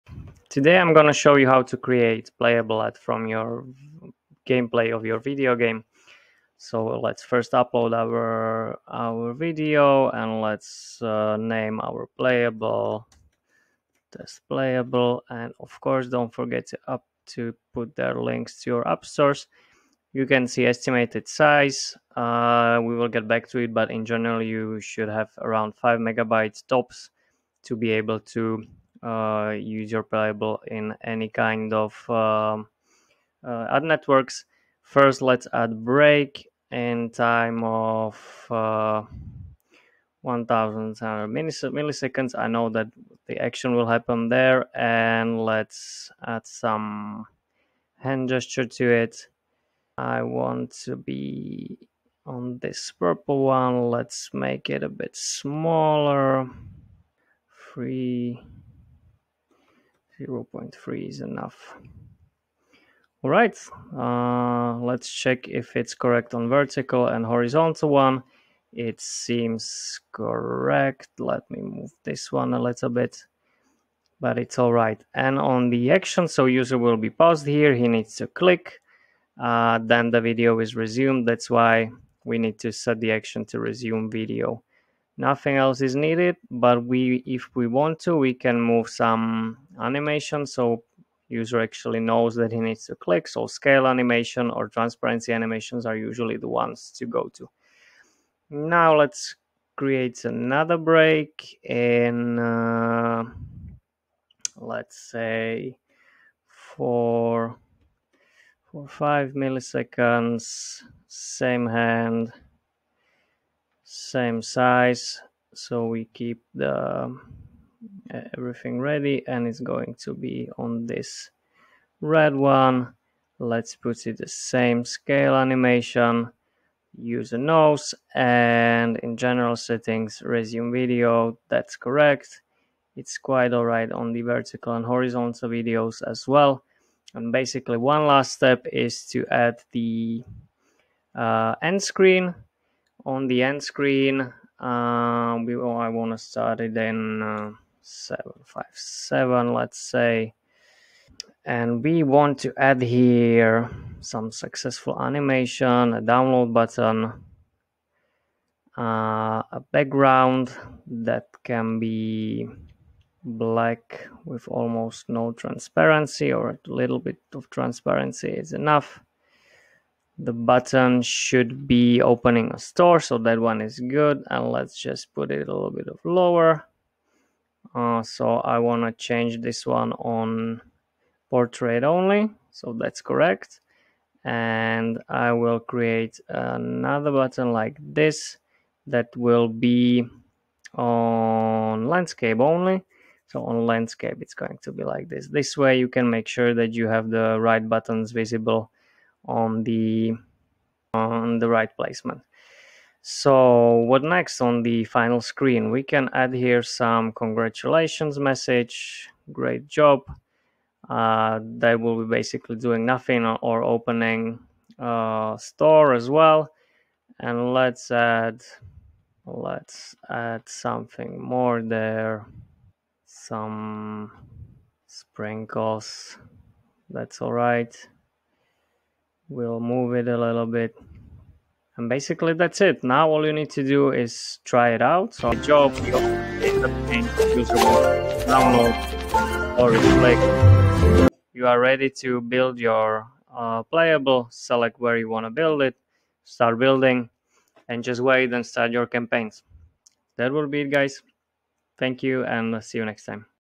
Creating Playable Ads from your gameplay video under 5 minutes !🎮
In this quick and easy tutorial, I'm going to show you how to turn your epic gameplay footage into a compelling playable ad—all in under 5 minutes! Whether you're a game developer looking to promote your game or just curious about the process, this guide is for you.

We'll cover everything you need to know to make your ad network-compatible, ensuring the playable size stays under 5 megabytes. Follow along as I walk you through:

Adding Breaks: Strategically place breaks to keep players engaged.
Incorporating Actions: Make your ad interactive and fun.
Animating Elements: Bring your ad to life with smooth animations.
Creating End Screens: Leave a lasting impression and call-to-action.
By the end of this video, you'll have all the tools you need to create a successful playable ad from your gameplay video. Don’t forget to try it out for yourself and share your experiences in the comments below!

Let's get started and transform your gameplay into a powerful marketing tool! 🚀